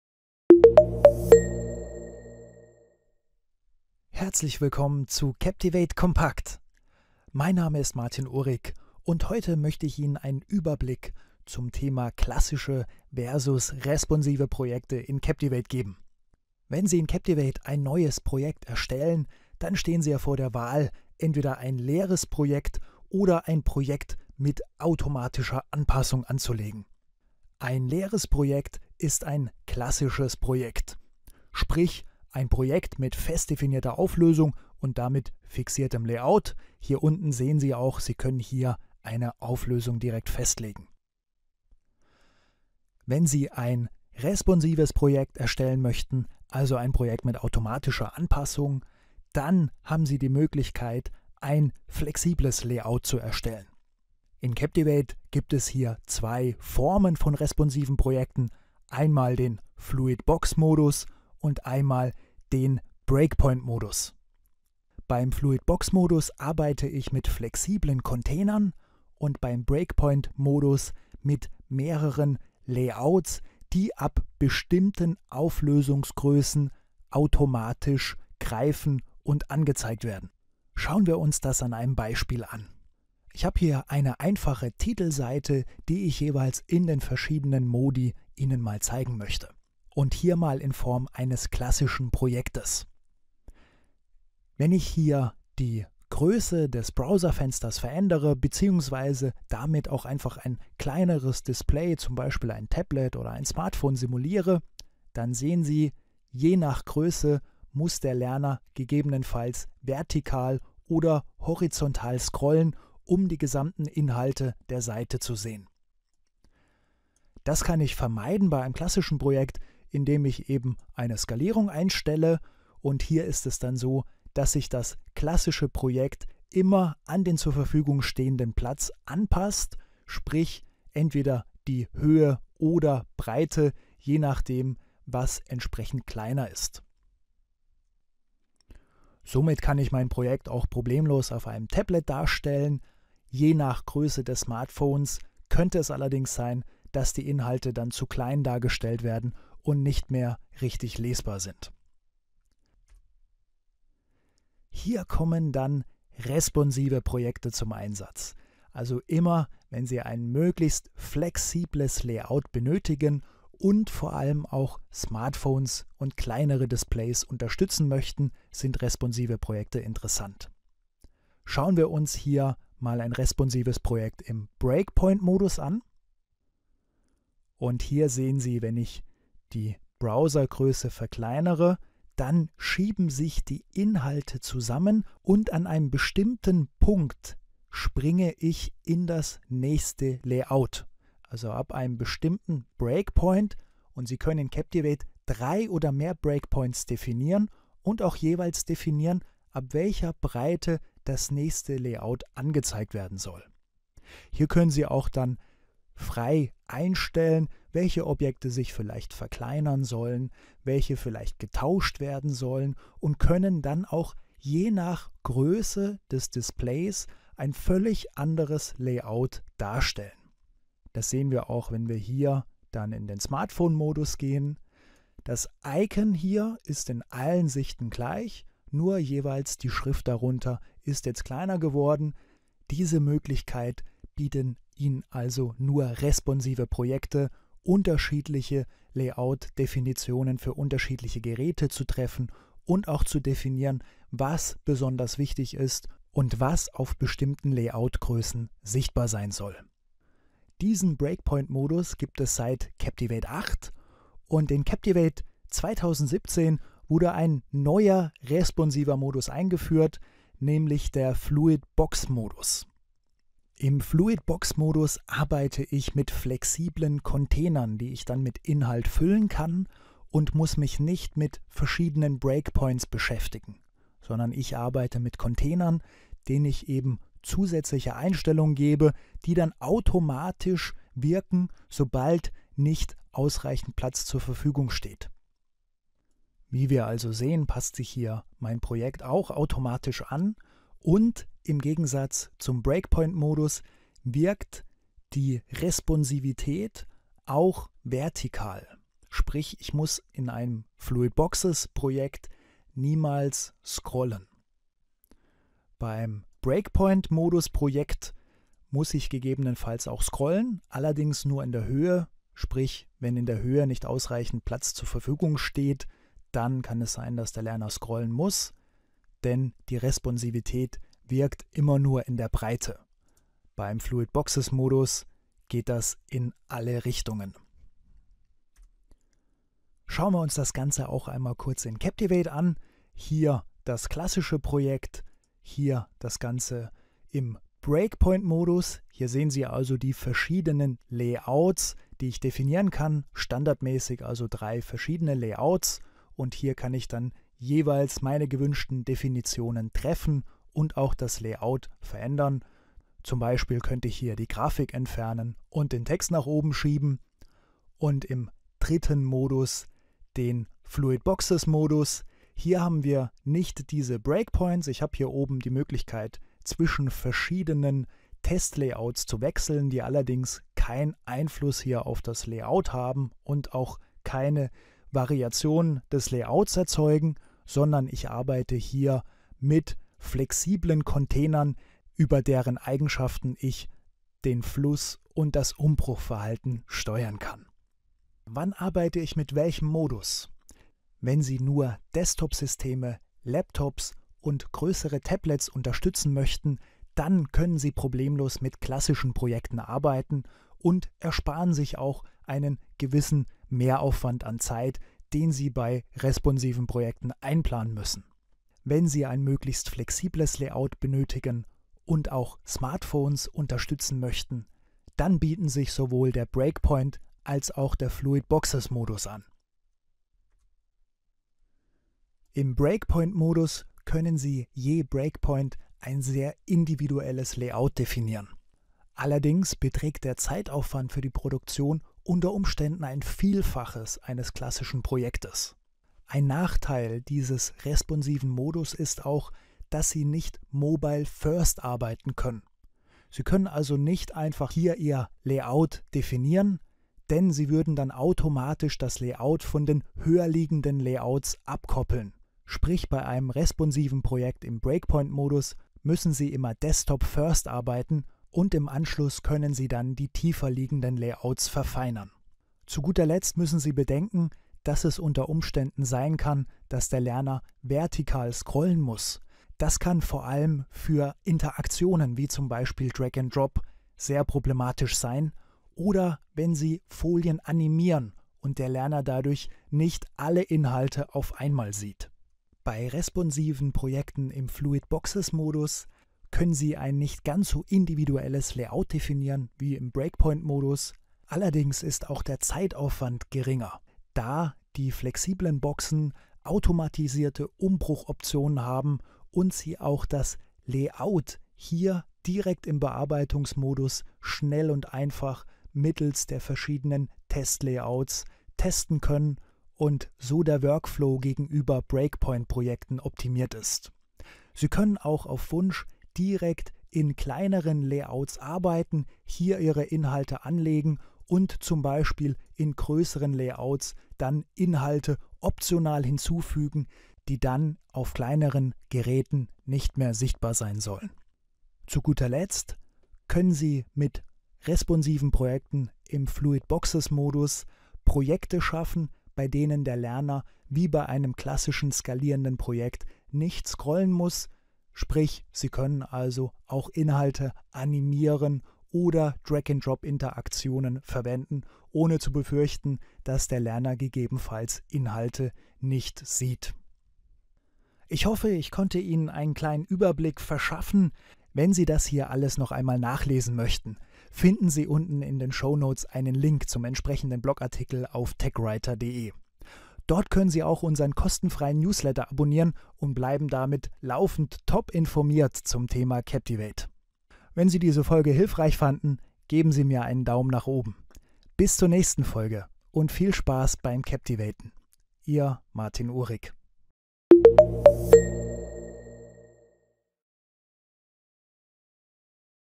Captivate kompakt: Klassische vs. responsive Projekte
In diesem Video erkläre ich Ihnen die Unterschiede zwischen klassischen und responsiven Projekten in Adobe Captivate 2017.

Adobe Captivate ist ein von Adobe Systems entwickeltes Autorenwerkzeug zur Erstellung von E-Learning-Einheiten und Screencasts. Mit Hilfe von Captivate können Sie ansprechende und motivierende digitale Lernmedien entwickeln. Dabei ermöglicht Ihnen das Tool auf einfache Weise, Texte, Bilder, Videos und Animationen mit Interaktivität zu verbinden und für verschiedene Endgeräte bereitzustellen.